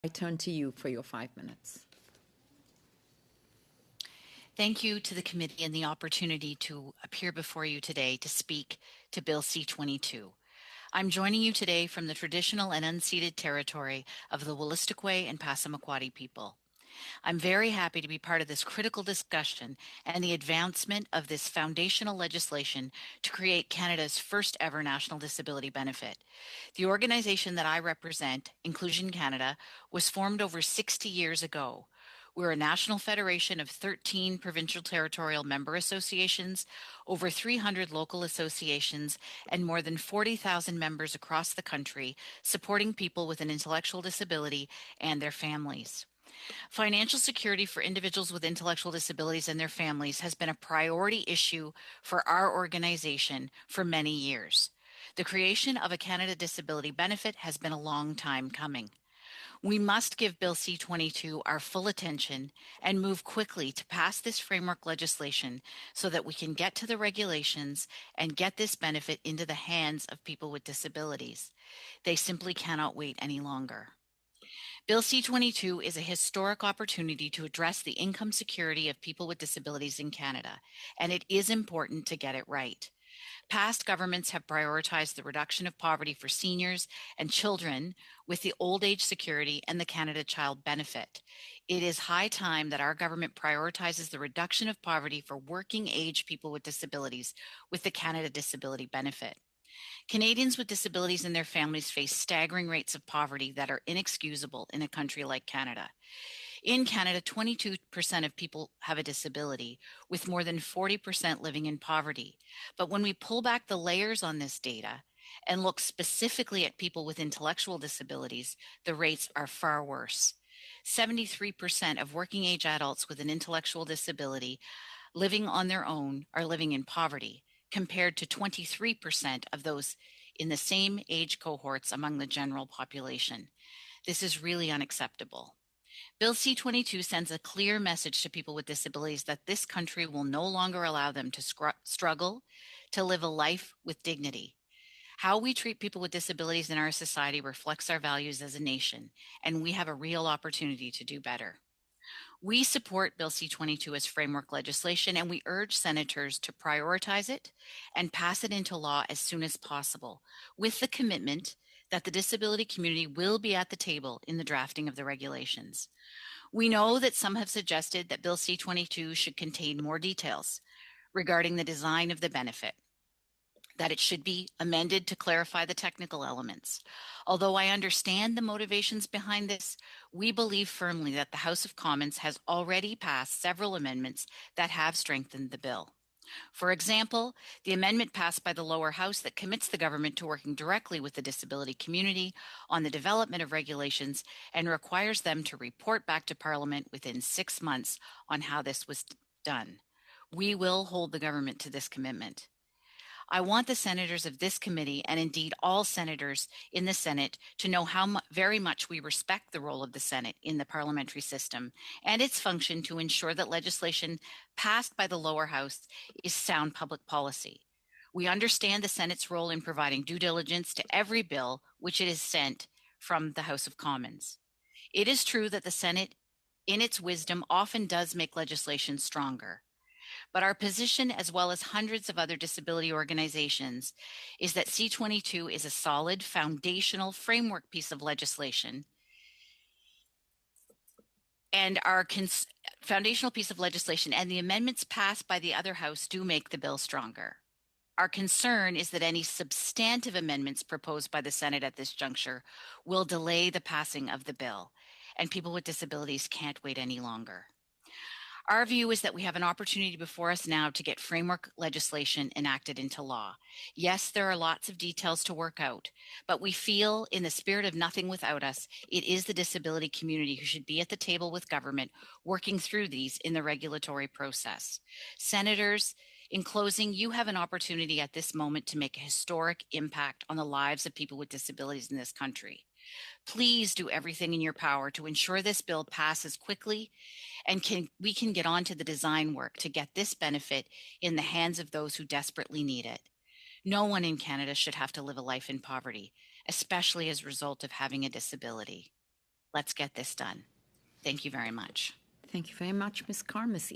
"Let's get this done." Inclusion Canada EVP Krista Carr appears at Senate on Bill C-22
Inclusion Canada Executive Vice President Krista Carr appears before the Senate of Canada with remarks for committee consideration of Bill C-22: the Canada Disability Benefit.